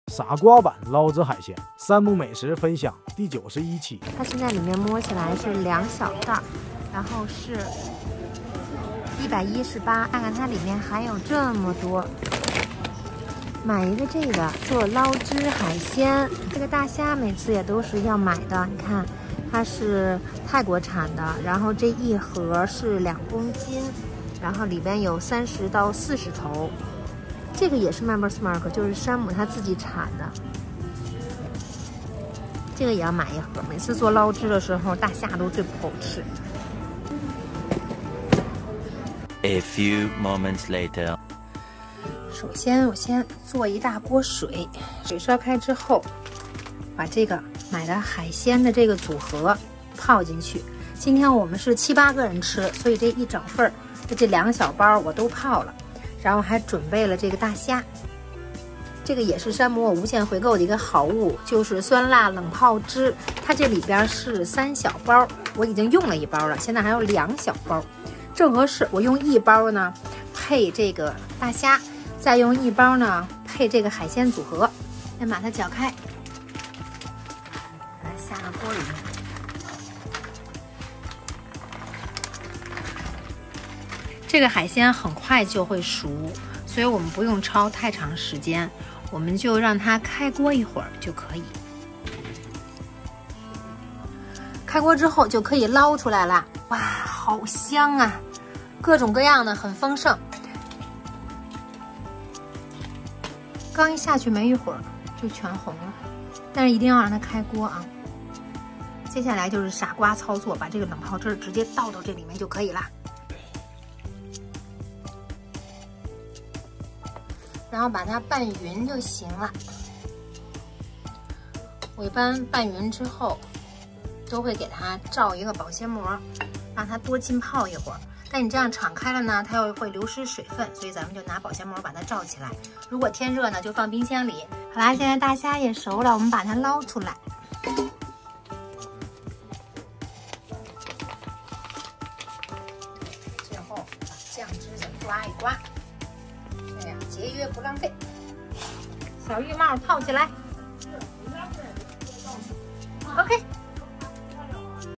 周奈迪Vlog | 山姆时令好物分享第91期 捞汁小海鲜安排上 | 记录生活 | 山姆 | 沃尔玛 | 超市 | Supermarket | 美食 | 快手菜 | 冷泡
很多家人都爱吃海鲜，今天用山姆的东西轻轻松松给大家做出一道捞汁小海鲜，轻松几步就能搞定，好吃得赞不绝口，如果你家里人也爱吃小海鲜，那赶紧做起来吧。